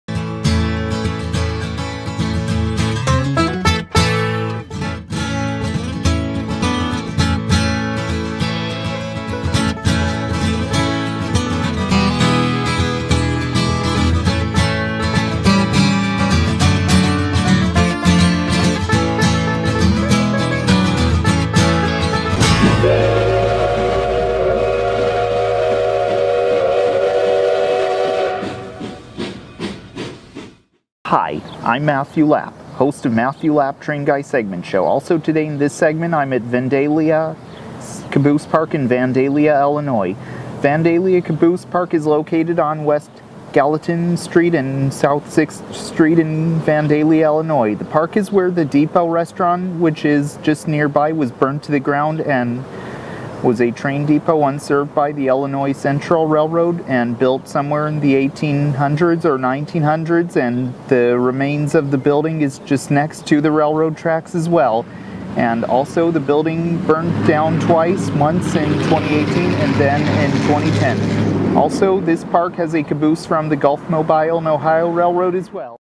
_Vandalia Caboose Park - Vandalia, IL_ Episode 294 (Beginning)
Vandalia Caboose Park
A small park in located near the remains of the Vandalia Depot
_MLTG_